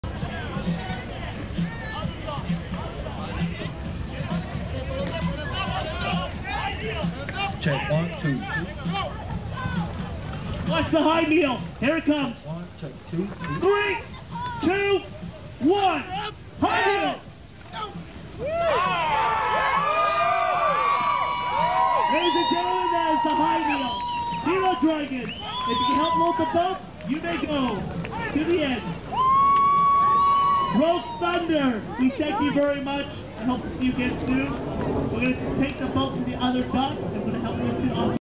Team DPW Knee High Paddling Stroke (Demo only)
Just having fun at the 2010 Arizona dragonboat festival. March 29, 2010